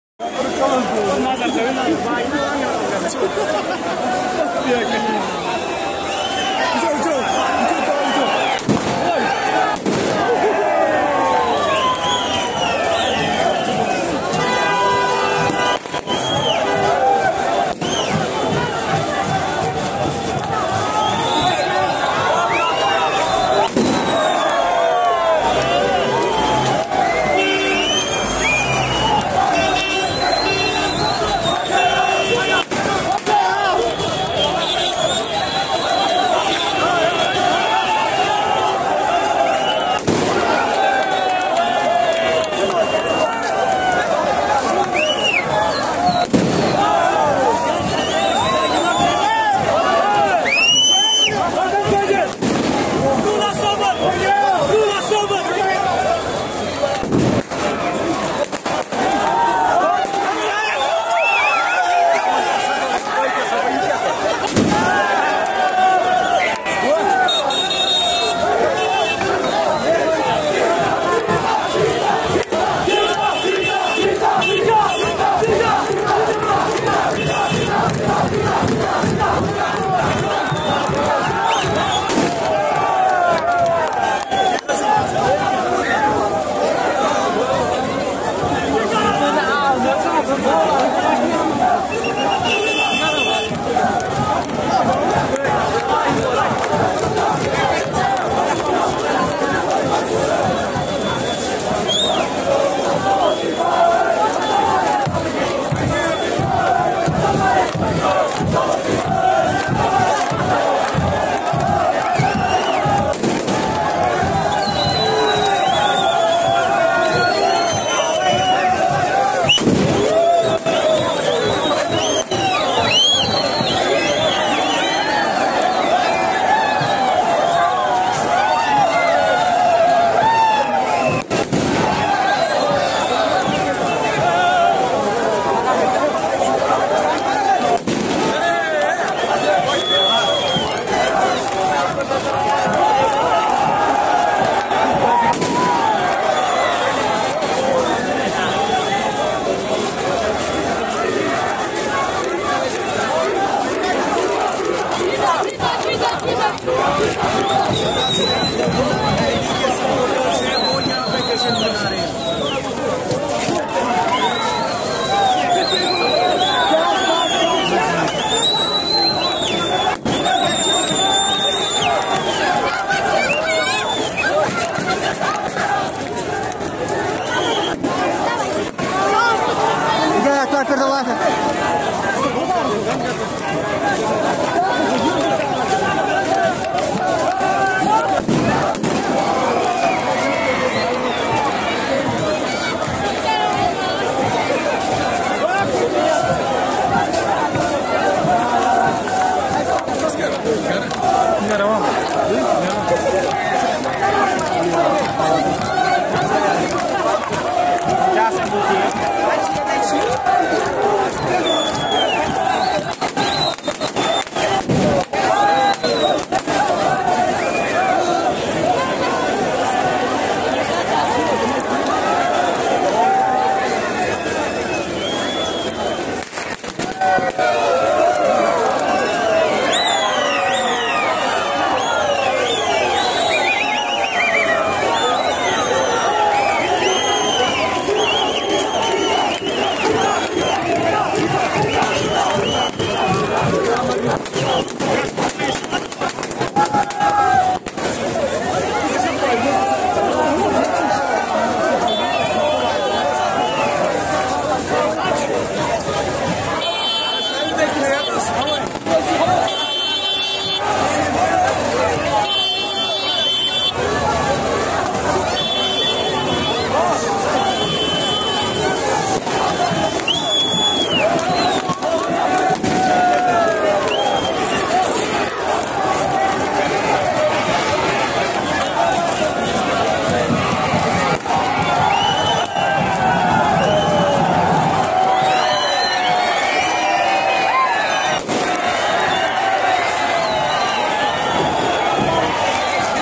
People from the Budgam main town celebrate the victory of the Aga Syed Ruhallah Mehdi
People from the Budgam main town celebrate the victory of  the  Aga Syed Ruhallah Mehdi  in Srinagar Parliamentary constituency seat.